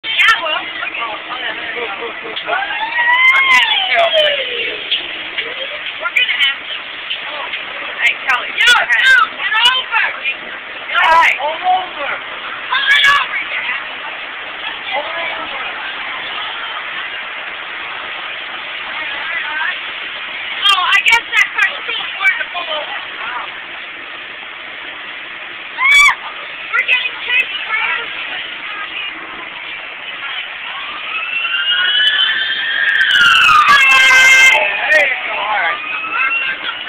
AMR Hot
AMR ambulance,southbound I-91,towards DT Hartford,CT